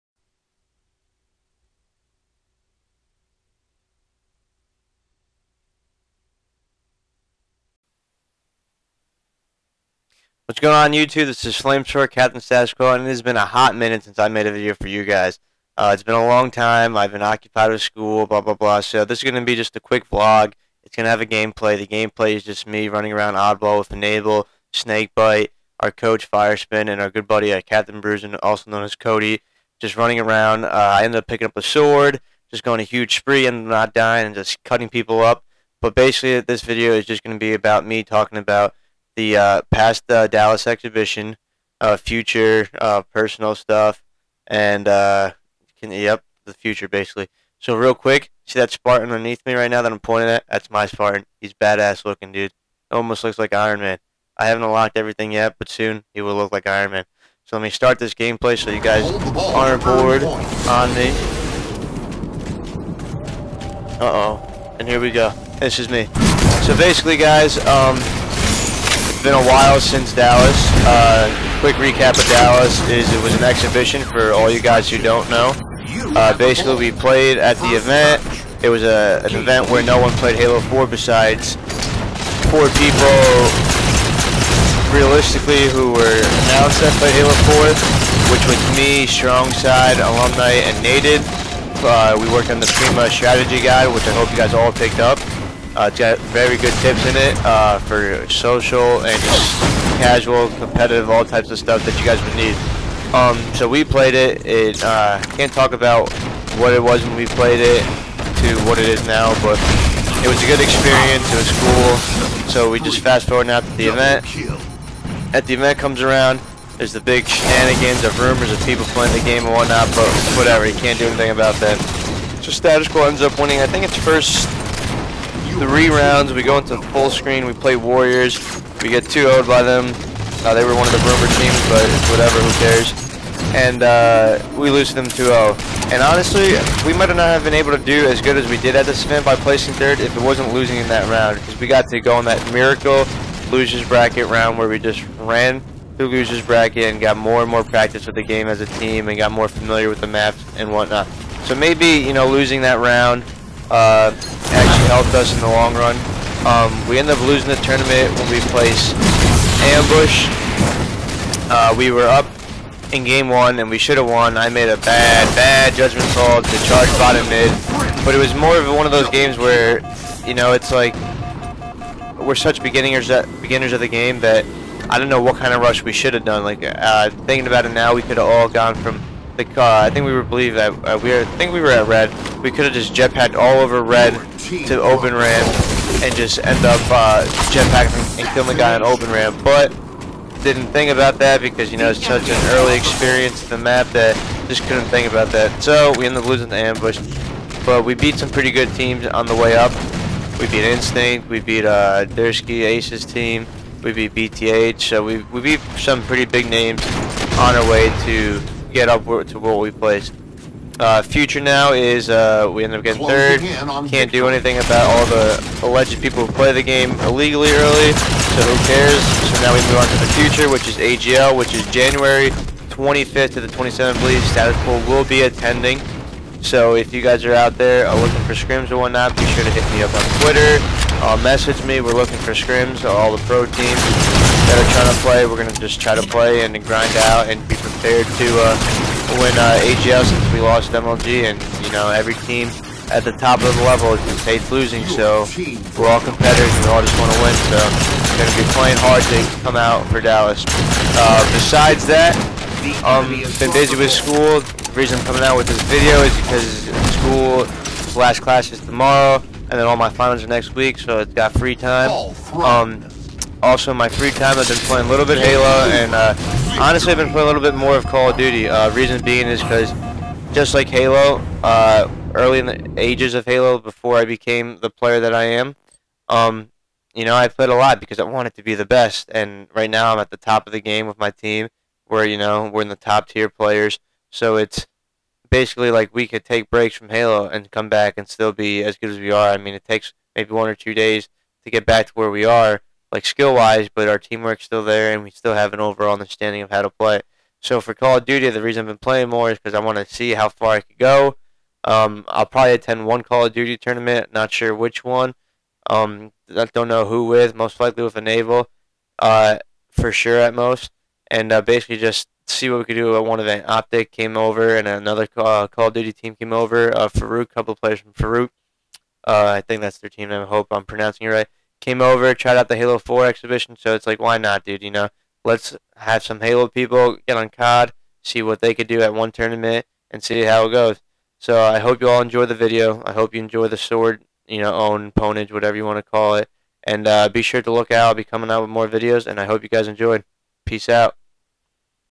Flamesword Vlog 8
Vlog that goes over MLG Dallas Exhibition, AGL Chicago Jan 25-27, being occupied with school, and play both Halo and COD.

Red Bull gives you wiiings!!!